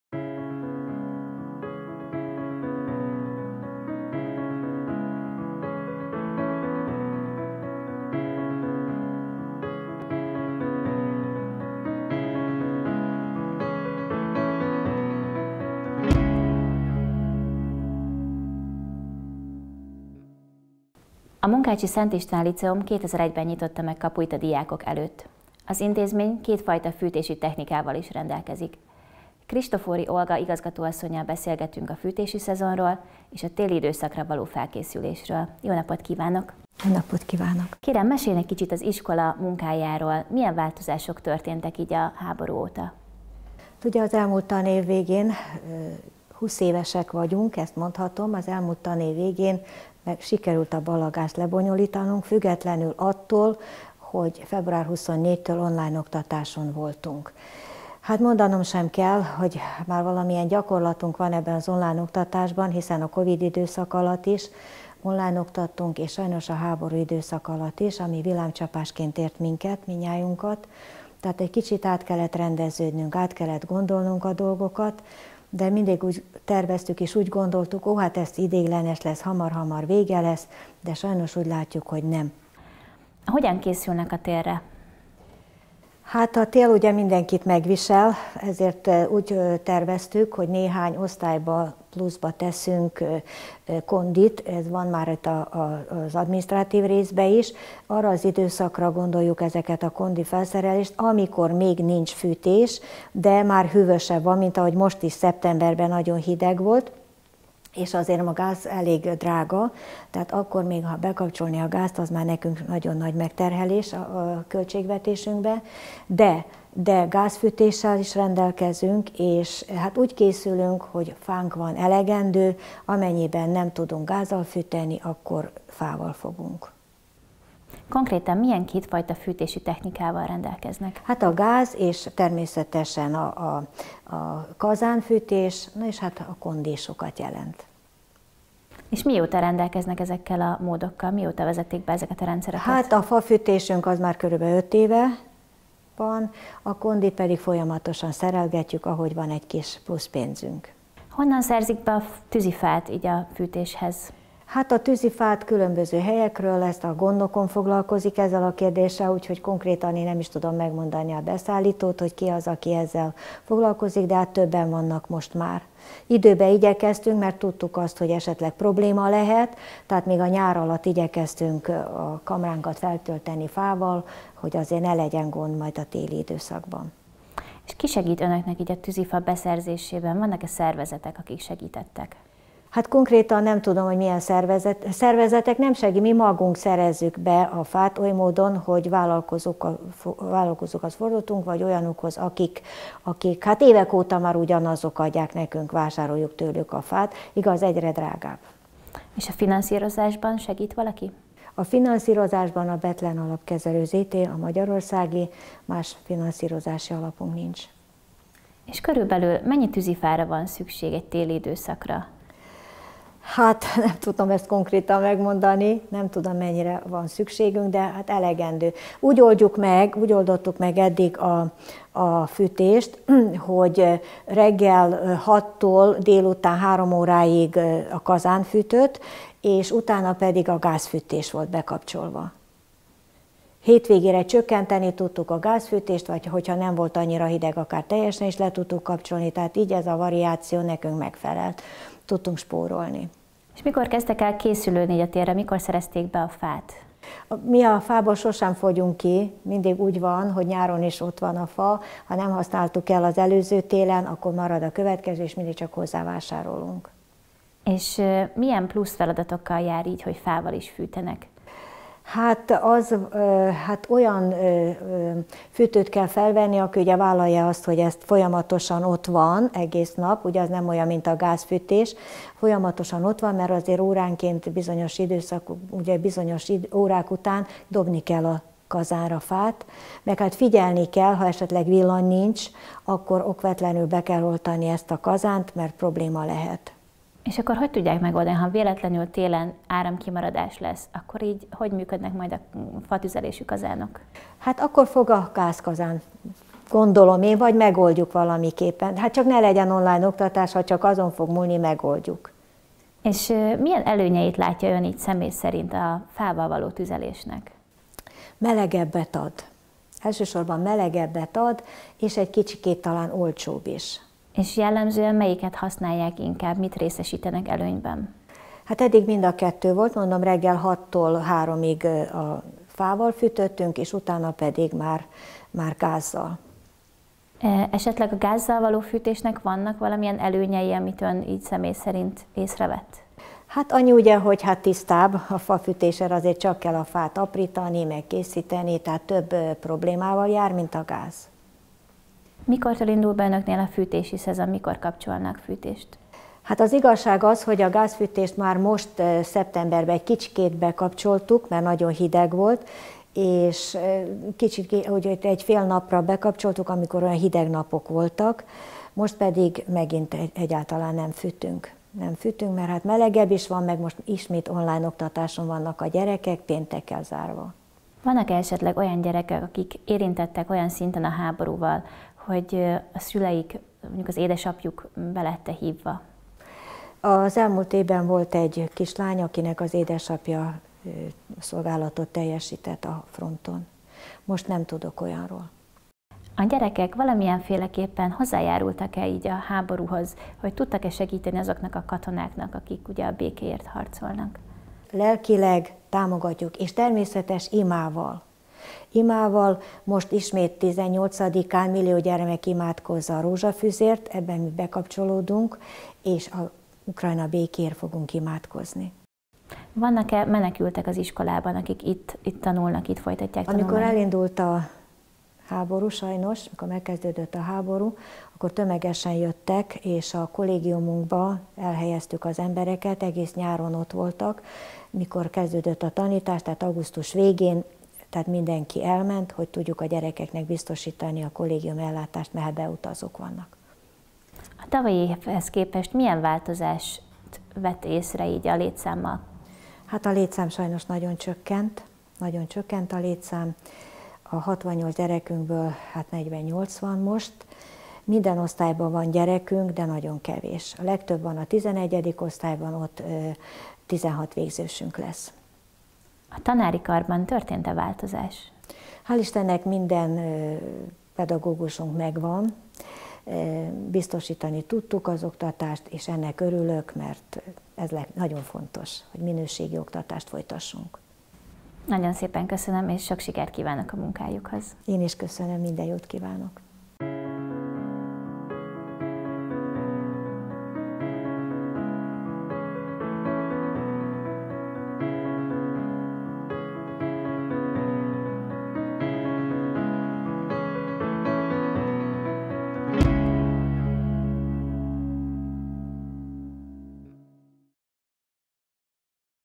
Підготовка до зимового періоду у Мукачівському ліцеї ім. Святого Іштвана | Спільна Мета
У випуску угорськомовного проєкту Спільна Мета (Közös cél) говоримо про підготовку до опалювального сезону та зимового періоду у Мукачівському ліцеї ім. Святого Іштвана. Спілкуємось з Ольгою Кріштофорі, директоркою закладу.

A Közös cél című magyar nyelvű projektünk adásában a Munkácsi Szent István Líceum fűtési szezonra és a téli időszakra való felkészülésről beszélünk Kristofori Olgával, az intézmény igazgatójával.